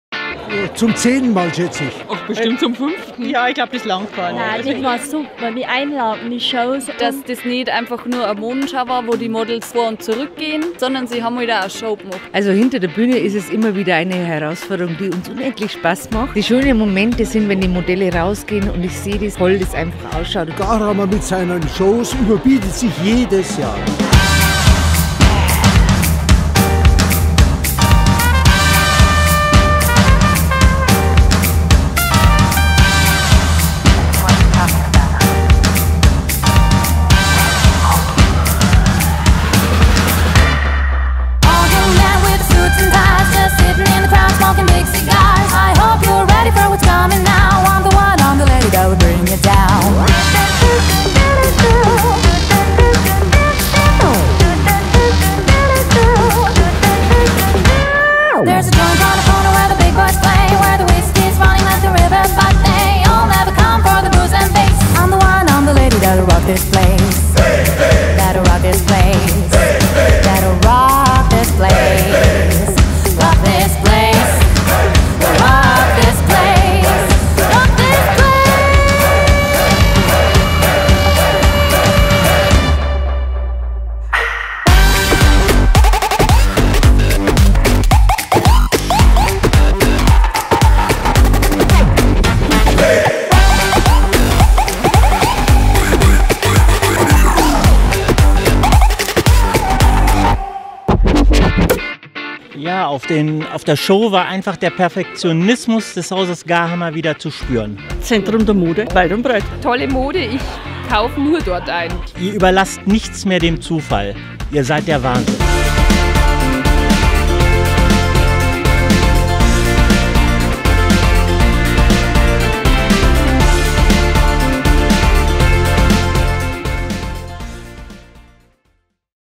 IHV Fashionteam Garhammer Modegala 2018
Das Modehaus Garhammer Waldkirchen begeistert jedes Jahr im September mit einer atemberaubenden Fashionshow by IHV Fashionteam..